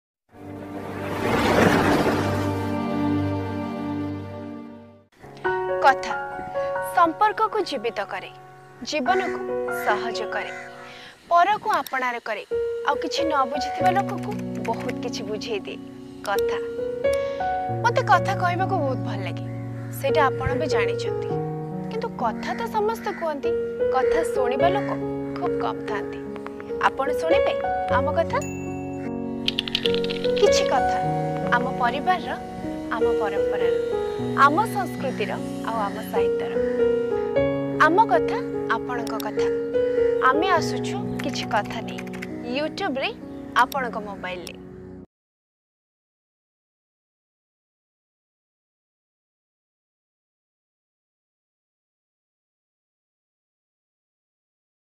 KATHA AMA ODISHA RA ll TRAILER II
"Katha Ama Odisha Ra"

In this YouTube channel we are going to see the Cultureal and Traditional Beauty of "ODISHA" Knowledge of the History, Geography, Culture and many  Tourist places situated here.